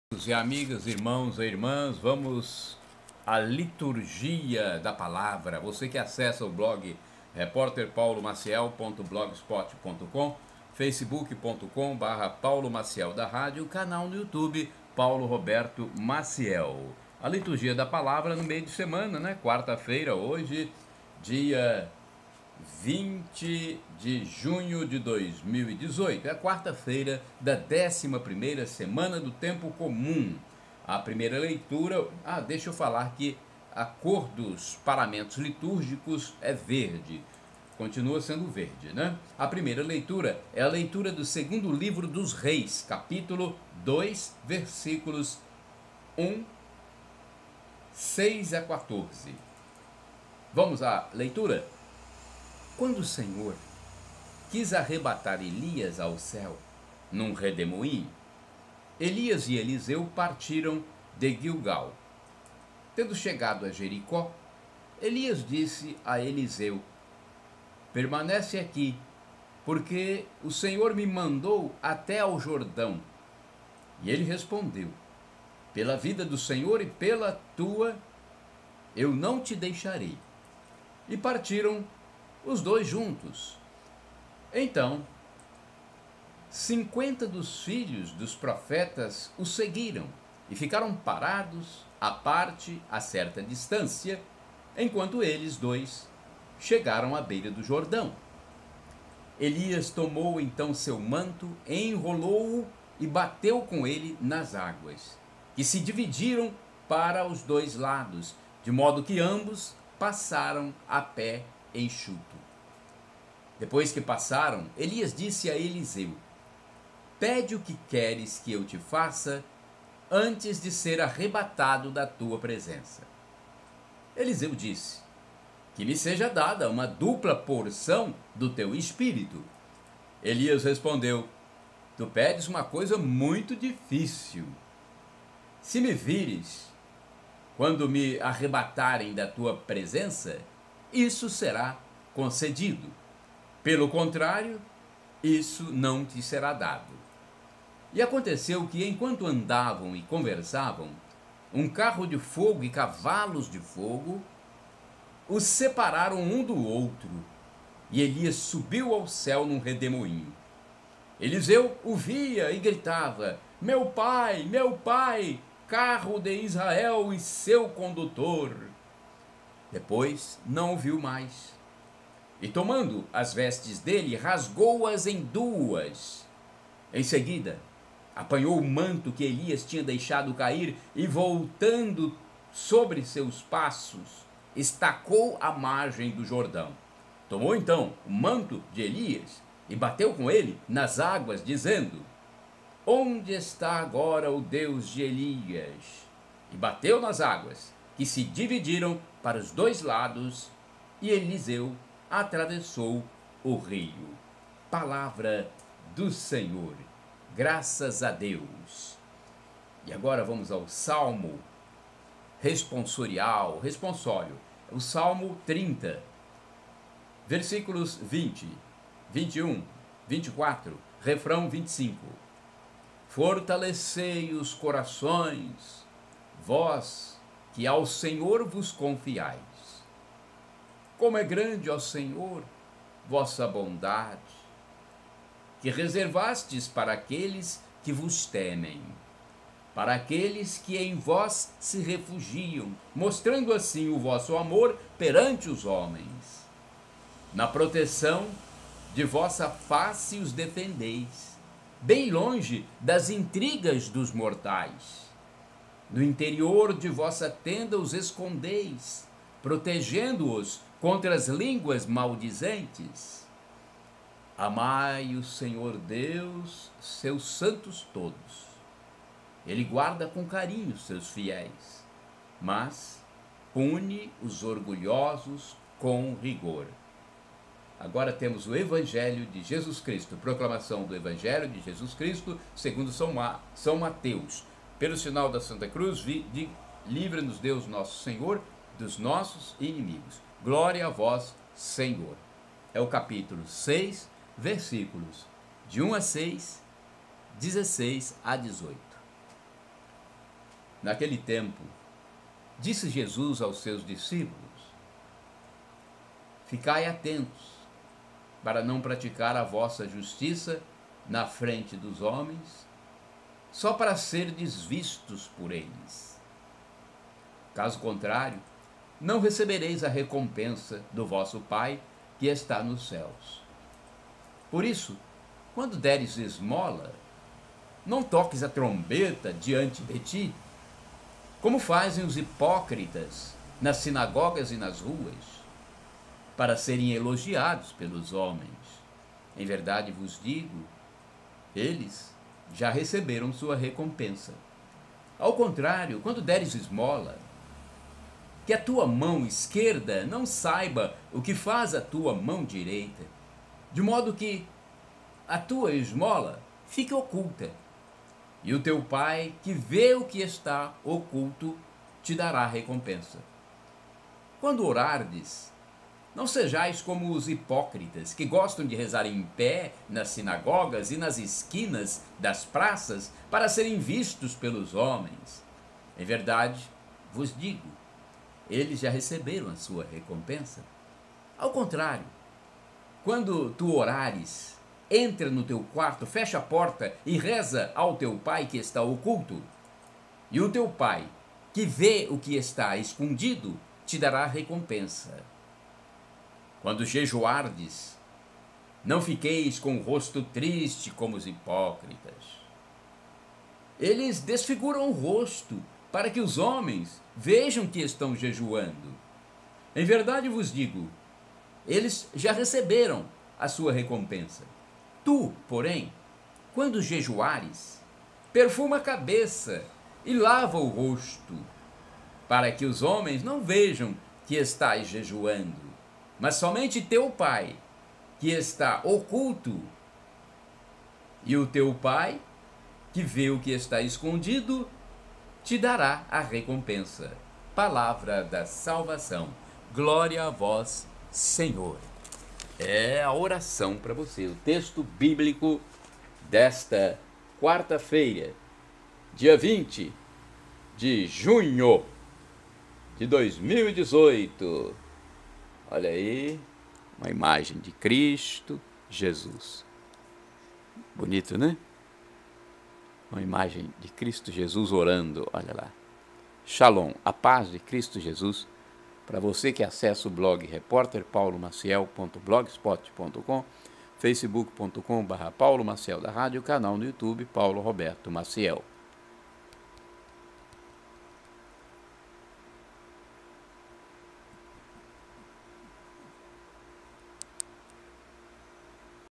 Liturgia, 4ªf, 20-6-2018. LOCUÇÃO DO RADIALISTA PAULO MACIEL - COLATINA
LITURGIA, 4ªF, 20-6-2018, NA LOCUÇÃO DO RADIALISTA PAULO MACIEL
QUARTA-FEIRA da 11ª Semana do Tempo Comum (20 de junho de 2018): Cor dos paramentos litúrgicos: VERDE. Primeira Leitura: Leitura do Segundo Livro dos Reis (2Rs 2,1.6-14): "Num carro de fogo, Elias subiu ao céu.". Salmo Responsorial: Salmo 30 (Sl 30,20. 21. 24 (R. 25)): "R. Fortalecei os corações, vós que ao Senhor vos confiais!" Proclamação do Evangelho de Jesus Cristo segundo São Mateus (Mt 6,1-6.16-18): "E o teu Pai, que vê o que está escondido, te dará a recompensa."   Fonte: SiteCNBB. Texto, filmagem e narração: Repórter, radialista e blogueiro Paulo Maciel, de Colatina/ES, para os sites paulo roberto maciel (canal no youtube.com), e reporterpaulomaciel.blogspot.com (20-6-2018)